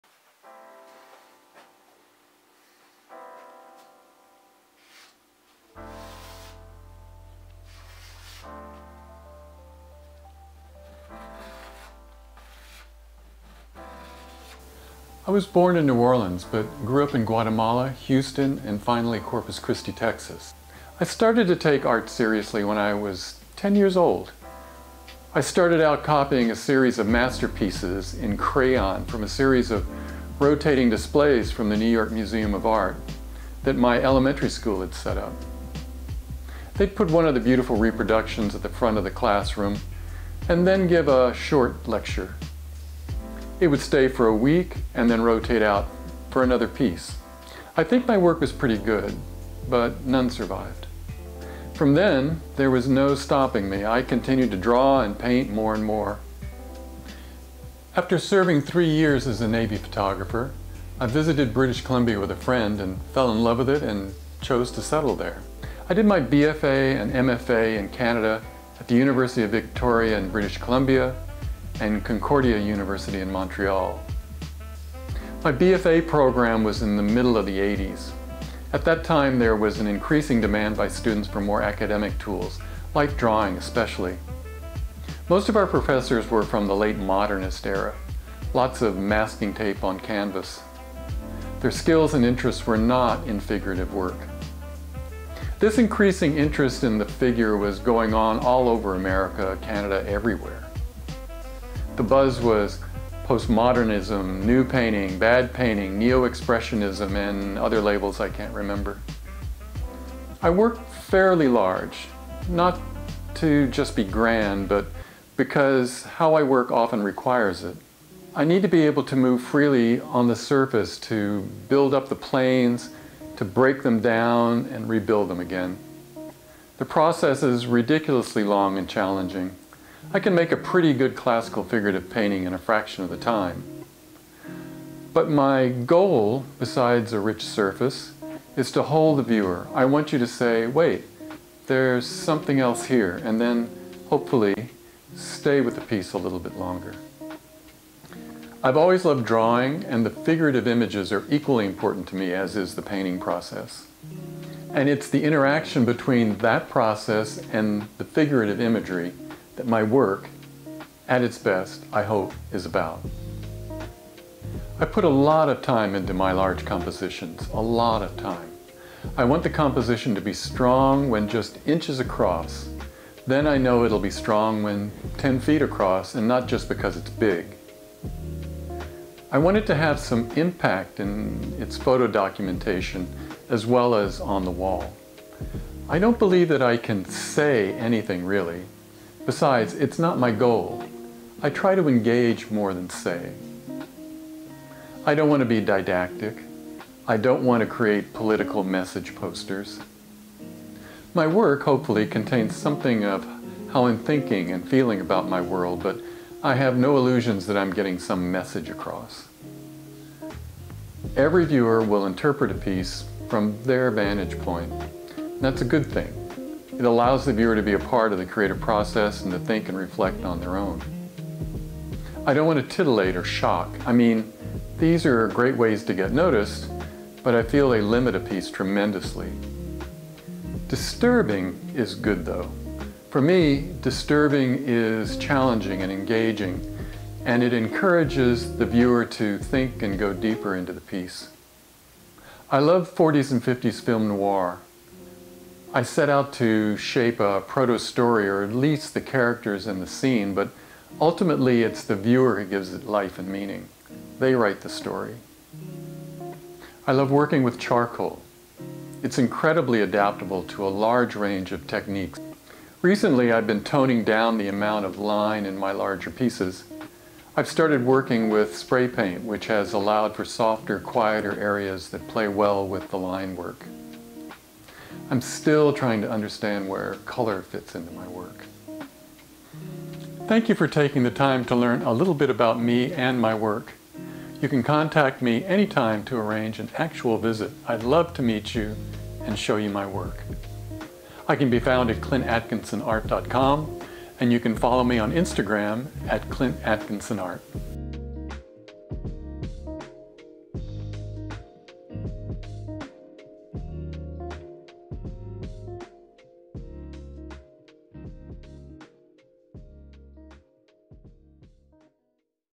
Clint Atkinson Virtual Studio Visit 2020
Clint Atkinson gives a virtual tour of his art studio for Big Medium's Austin Studio Tour 2020.